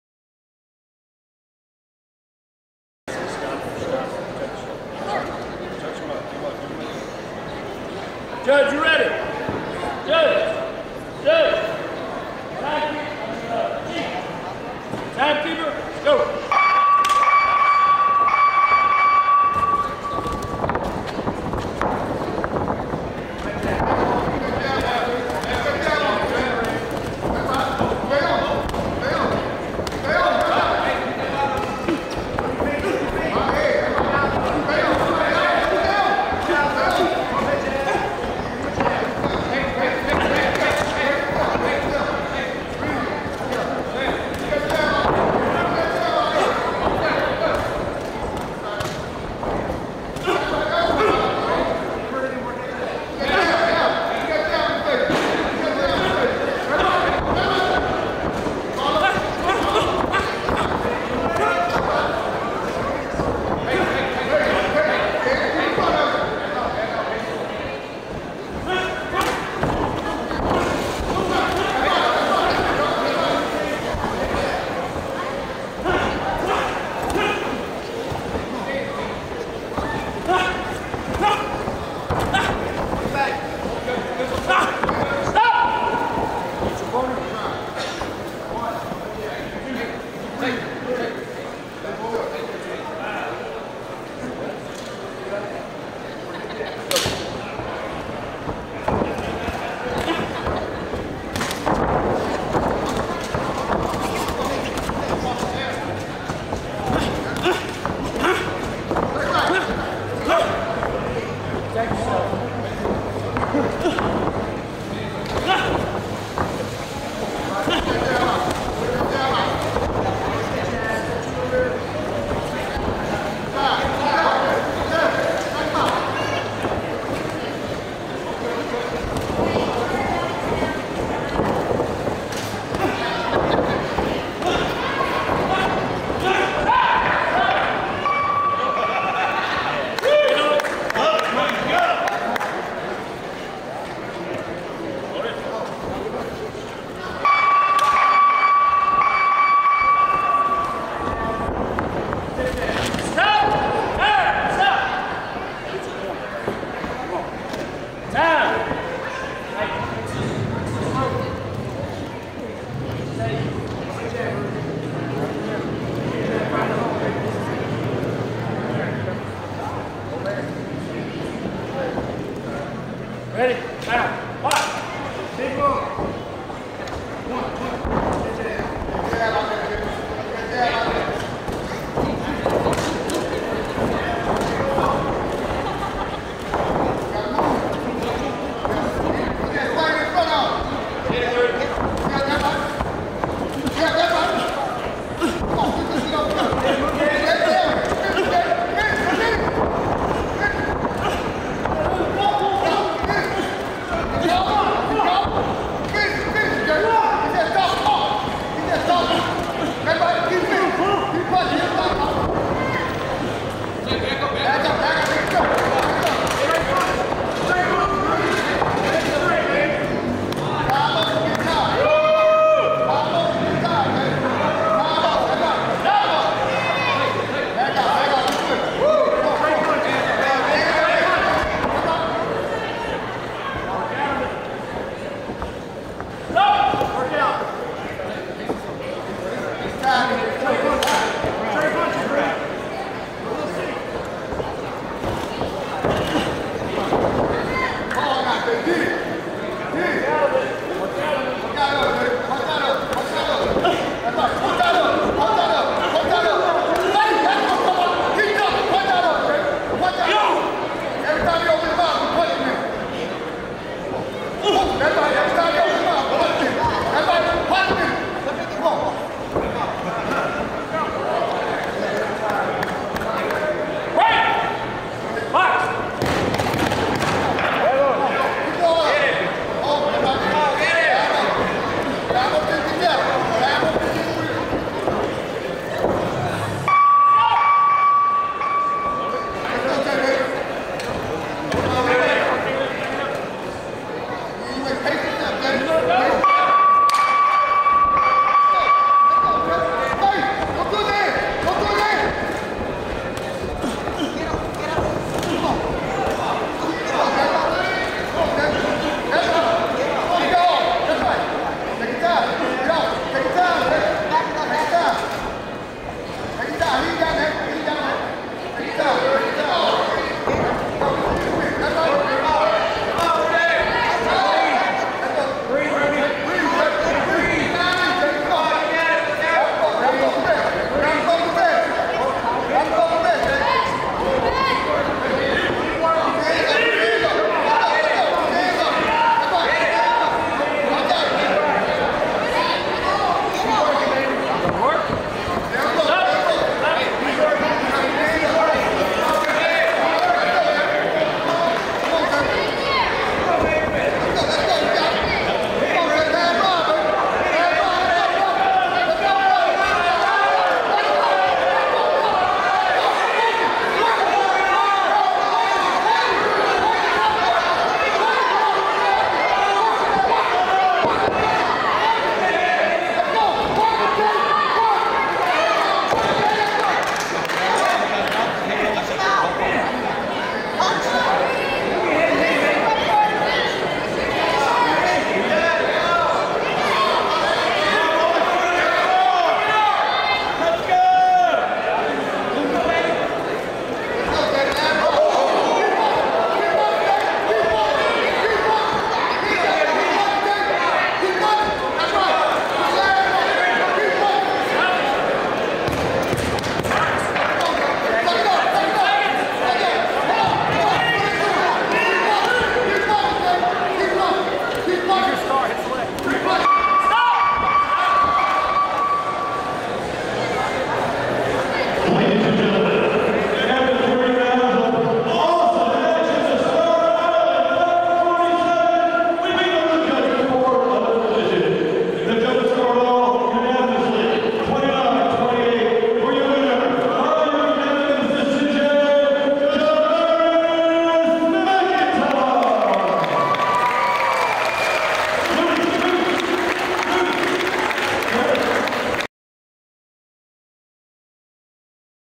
Island Fights 47: Prieto vs McIntyre
Amateur Fights at the Escambia County Equestrian Center, Best Viewed In 1080p HD